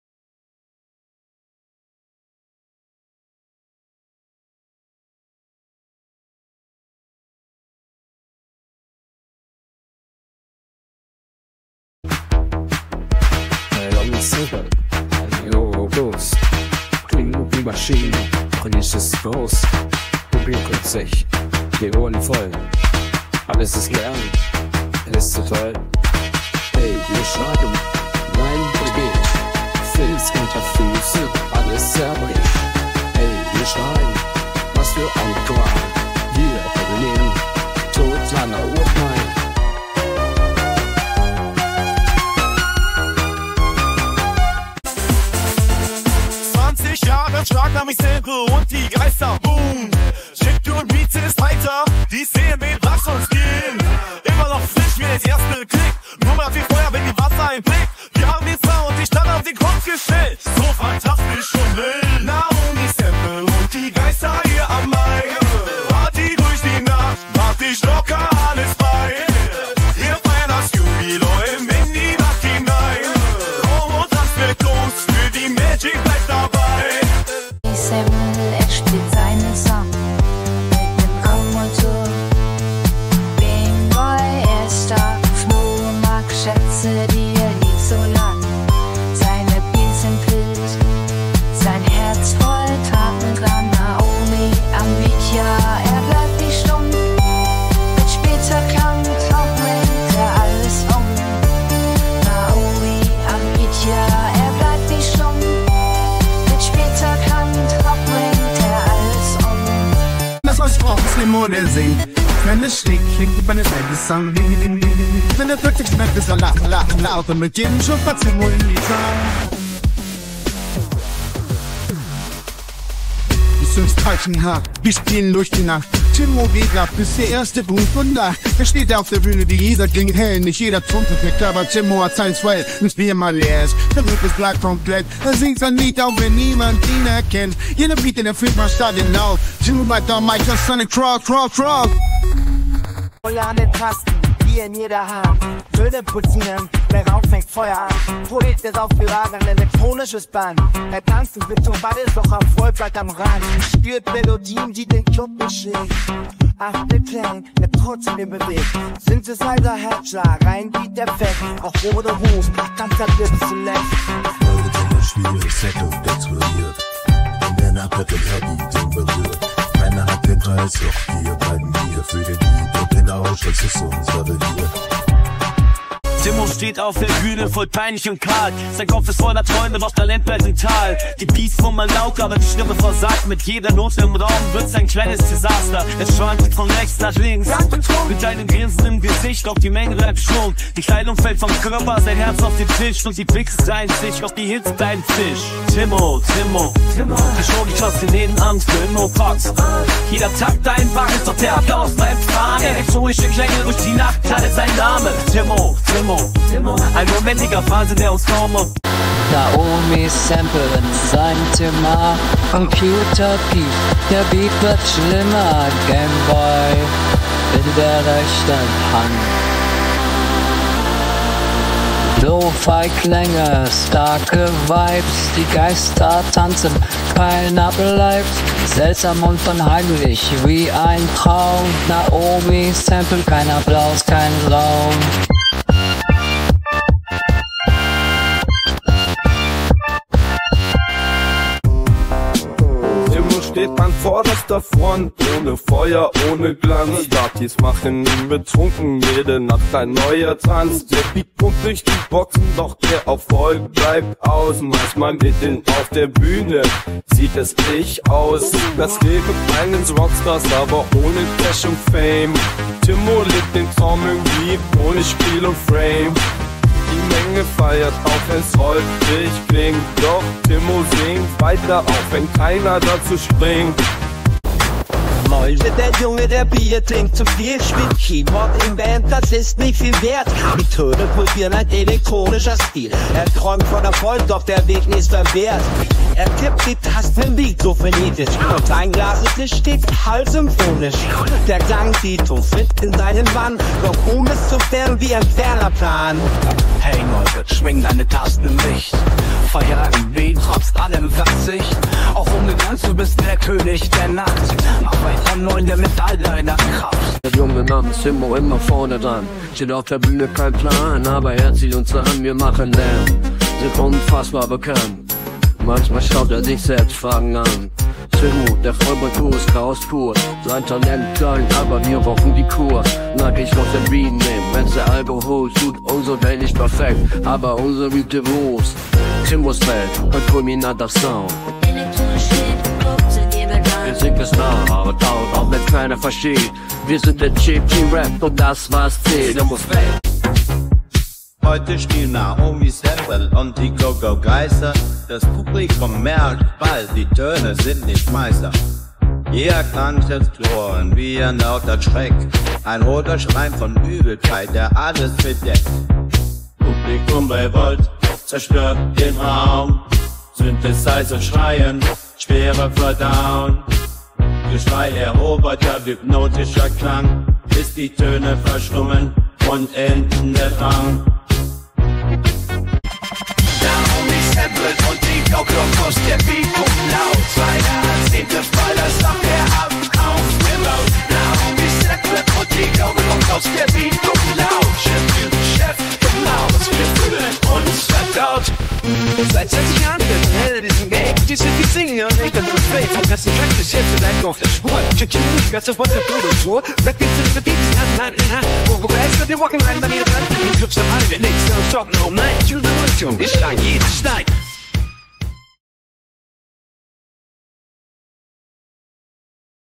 KI composes songs about Naomi Sample & the Go Go Ghosts
We present 12 song-skits about our band composed by artificial intelligence. We fed the machine with prompts for the AI-audio-tool udio.com like this:

"A song in germann language im stil von "Kraftwerk" with very old synthesizer sounds, the lyrics should be about a band called "Naomi Sample & the Go Go Ghosts" and the audience finds them terrible must puke while listening to the band and the audience is frustrated and destroys everything in the venue, electronica, robot sounds, chiptunes, 80ties."

Have fun with the results!